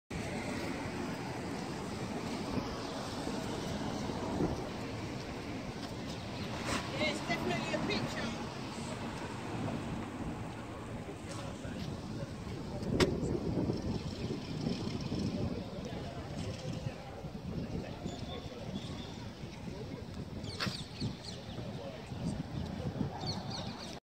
Buckingham Palace with Faux Tower Covered with Tarpaulin
And added tower on the end of the palace and and the lower level covered with a printed tarpaulin too. Only two armed police at the entrance closest to the hidden exterior parts of the palace.